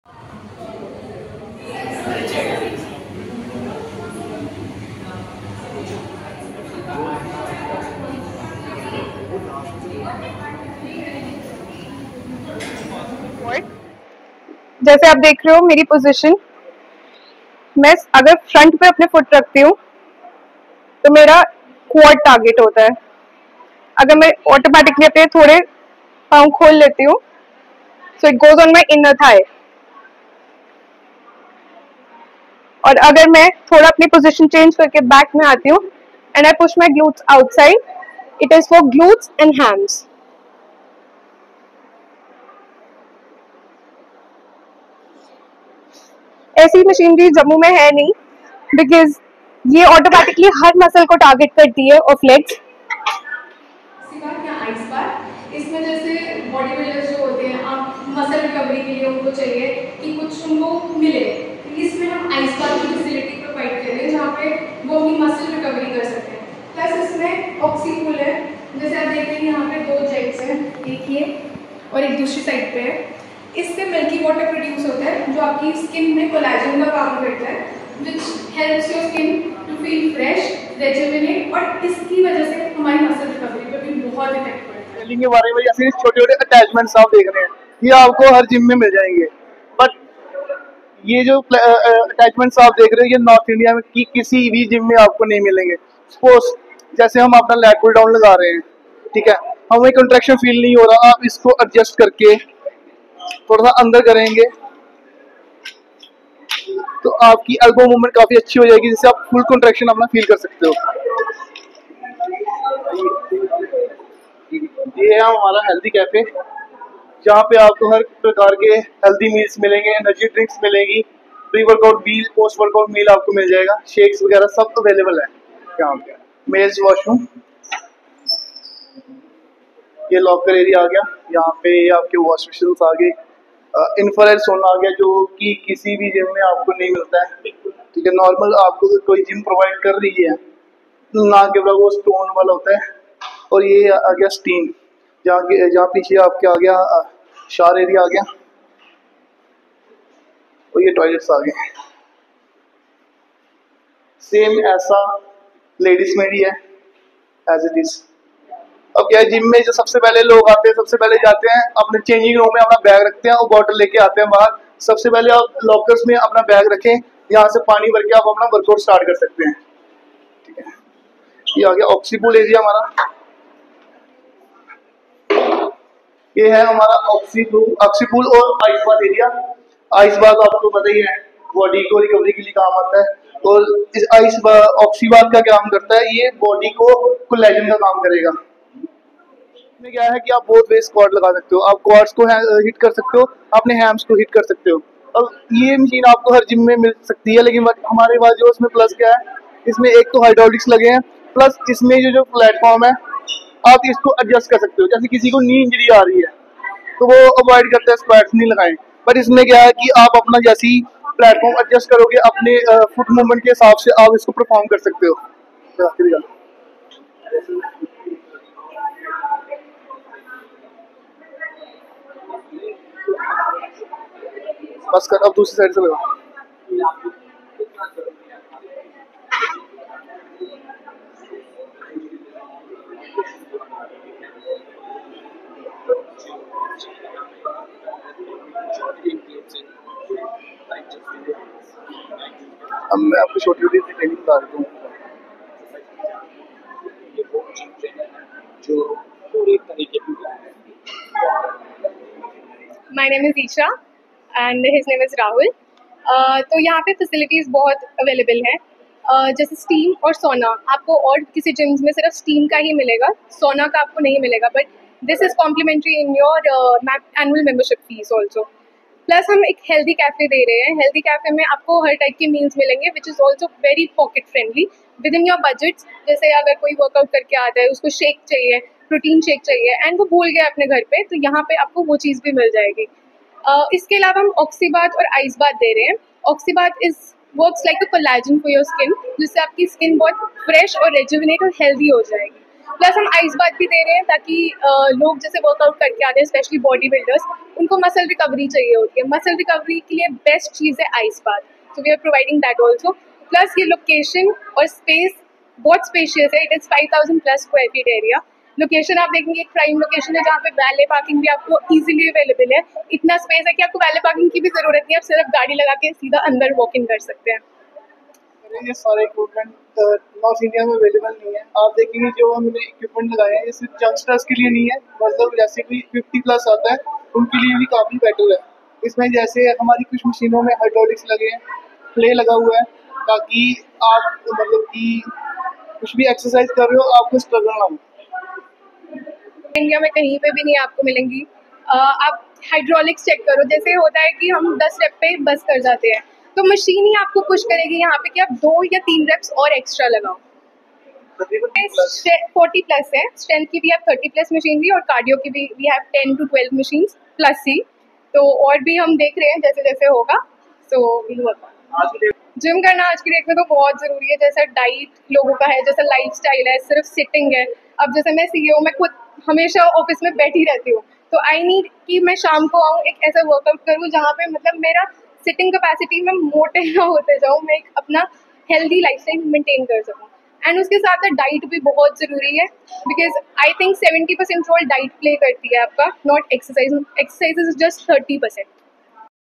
Ground Zero Gym Launches: Redefining Fitness with State-of-the-Art Facilities.
Location: Choudhary Towers, Sec 1-A, Trikuta Nagar, Jammu.
Timings: 6am to 11pm
Hydraulic machinery for the first time in North India.
Facilities of Oxy Bath/Ice Bath, Steam, Sauna, Healthy Cafe, outdoor lounge, valet parking.
Contact persons: Rahul Thapa and Isha Singh